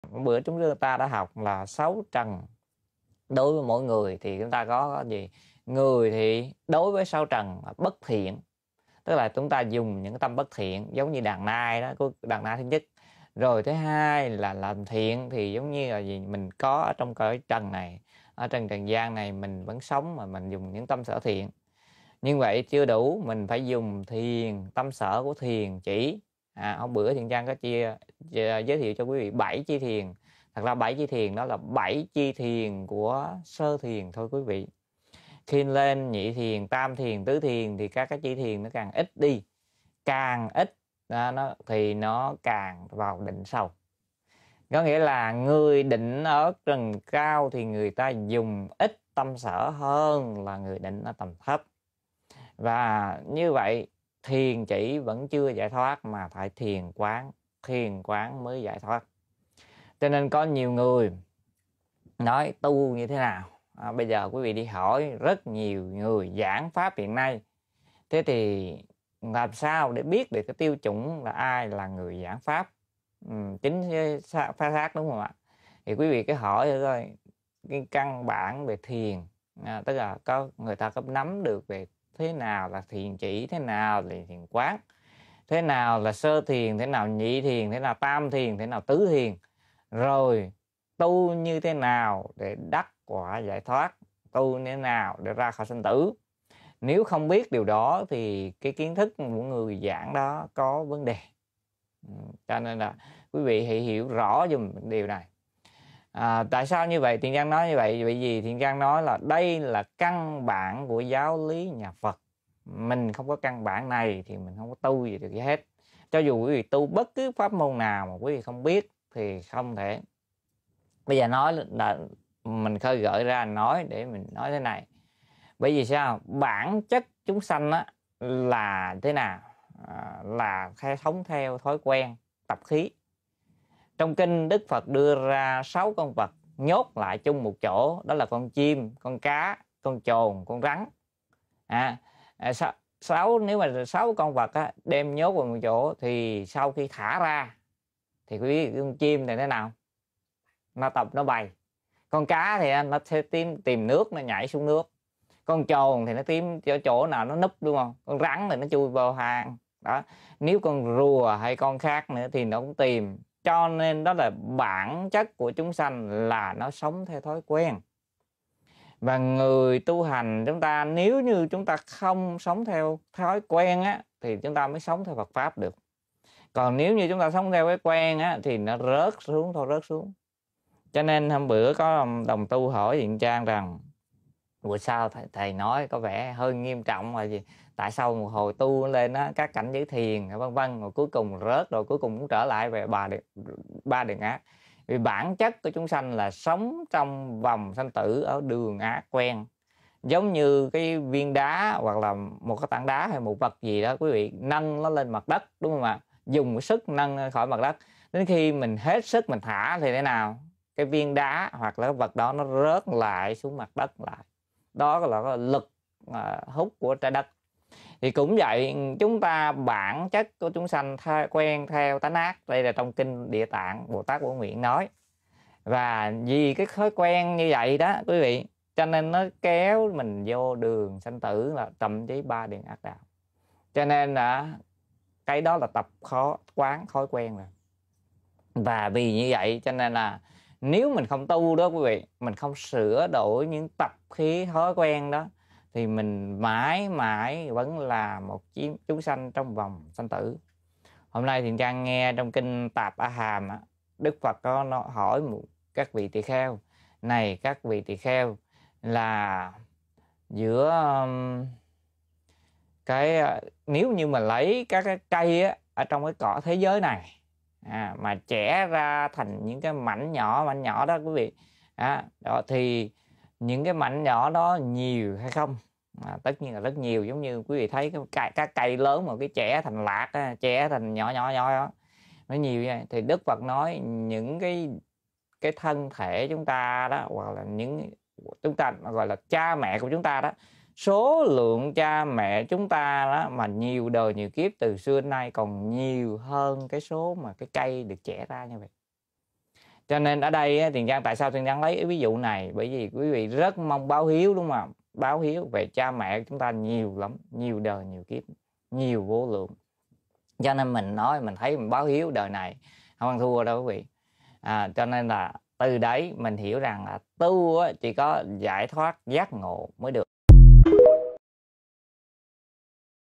BẢN CHẤT CỦA CHÚNG SANH LÀ SỐNG THEO THÓI QUEN VÀ TẬP KHÍ. NGƯỜI GIẢNG PHÁP PHẢI NẮM RÕ GÌ
_ BẢN CHẤT CỦA CHÚNG SANH LÀ SỐNG THEO THÓI QUEN VÀ TẬP KHÍ
_ TU HỌC MÀ KHÔNG SỬA ĐỔI THÓI QUEN VÀ TẬP KHÍ THÌ MÃI MÃI CHỈ LÀ MỘT CHÚNG SANH TRONG VÒNG SANH TỬ
_NGƯỜI GIẢNG PHÁP PHẢI NẮM RÕ NHỮNG ĐIỂM NÀO?

Chúng con cung kính trích lục từ A Tỳ Đạt Ma Câu Xá Luận tập 6
Giảng giải: Thầy Thích Thiện Trang
NAM MÔ A MI ĐÀ PHẬT